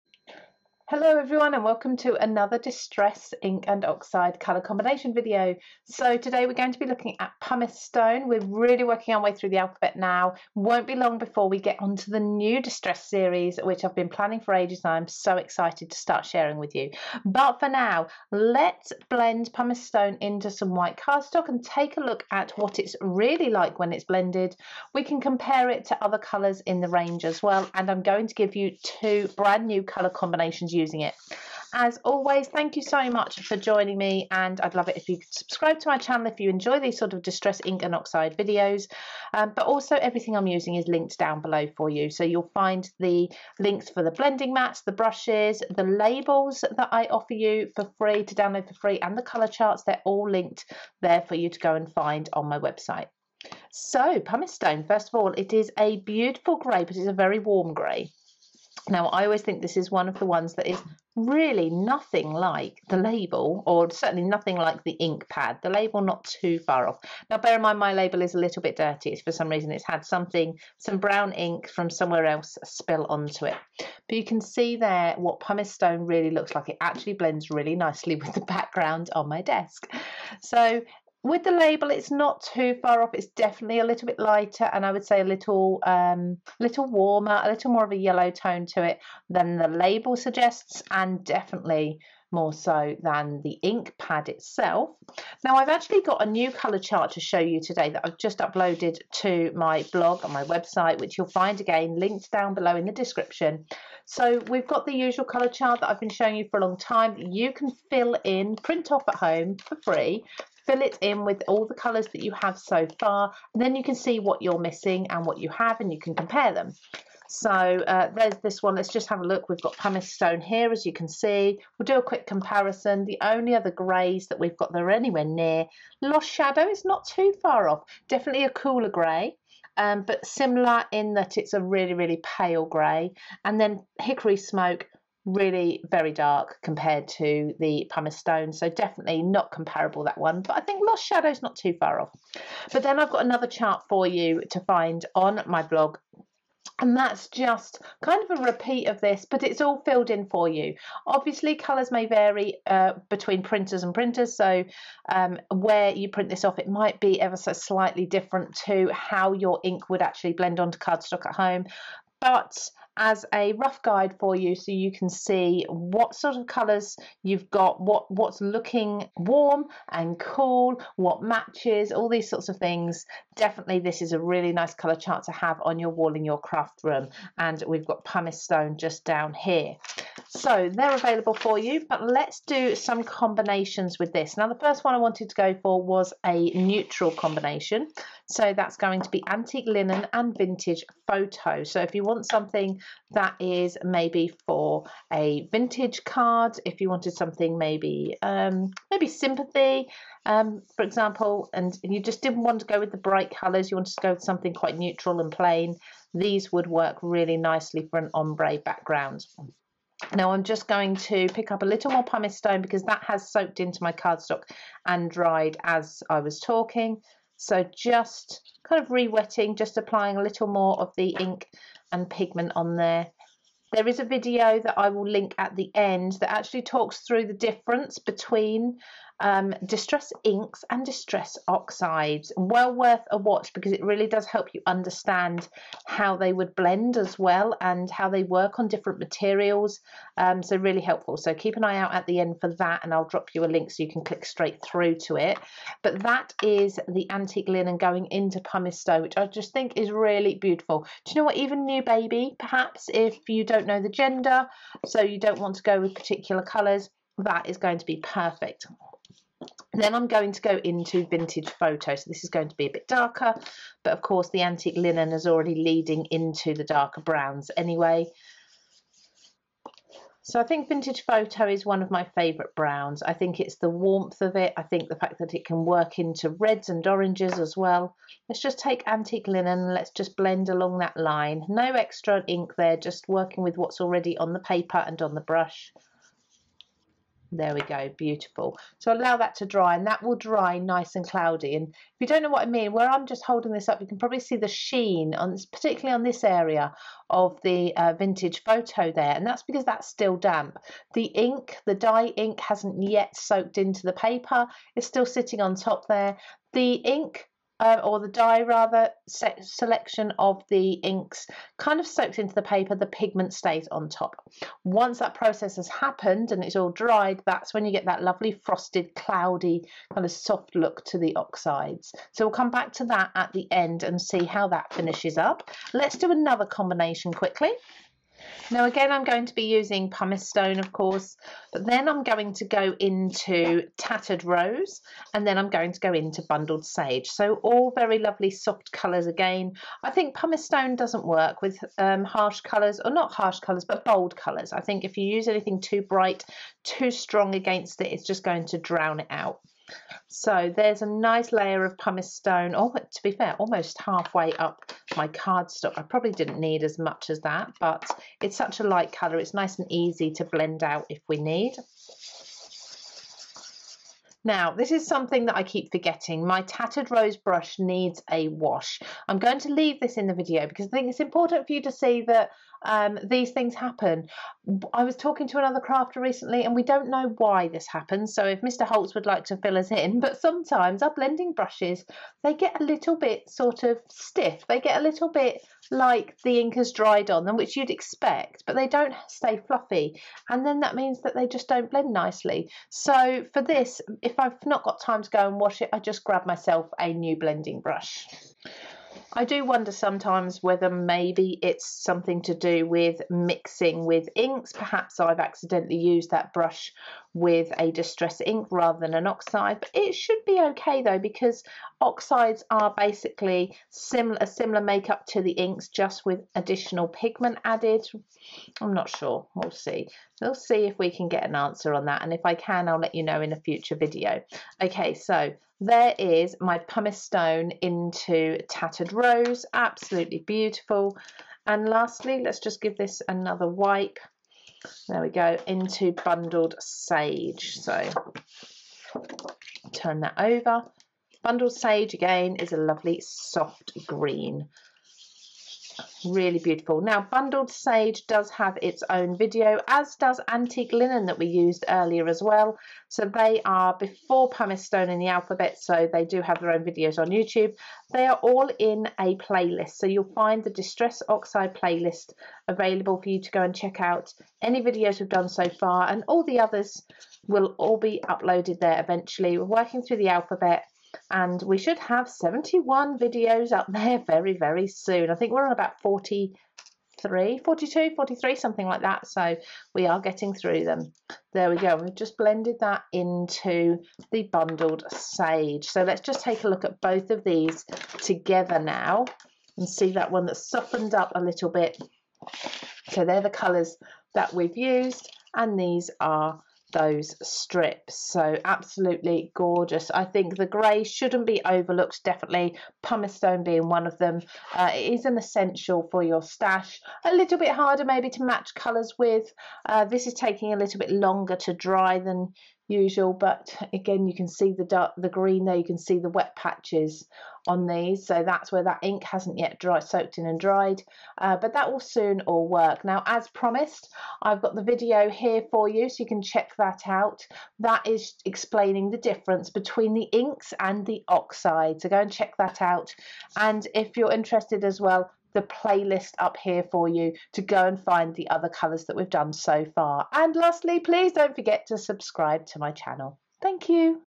DISTRESS OXIDE COLOUR COMBINATIONS - Pumice Stone
Welcome to this series looking in-depth at each of the Ranger Tim Holtz Distress Ink & Oxide colours and discovering colour combinations and ink blending techniques with each.

This tutorial is looking at Pumice Stone plus two gorgeous colour combinations using it.

All videos in this series will be uploaded alphabetically, according to the colour name. Please see the Distress Oxide Colour Combinations playlist here ➡️ for more.

Other Distress Oxide colours used:
* Antique Linen
* Tattered Rose
* Bundled Sage
* Vintage Photo

You will find the Distress Ink & Oxide colour chart and labels FREE for you to download on my website here

For more paper craft tutorials, tips and projects;